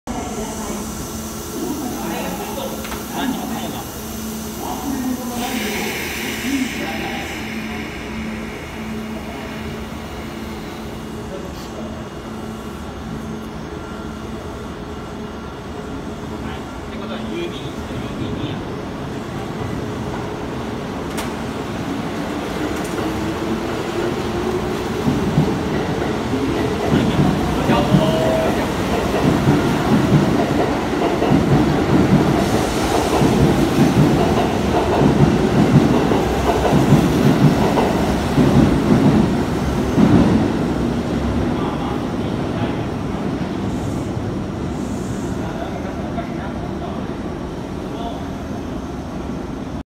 【近鉄21000系】UL02 名古屋行き特急アーバンライナー 発車 Limited Express Urban Liner bound for Nagoya
8時30分発
0108レ

撮影：大阪難波駅（2022/10/09）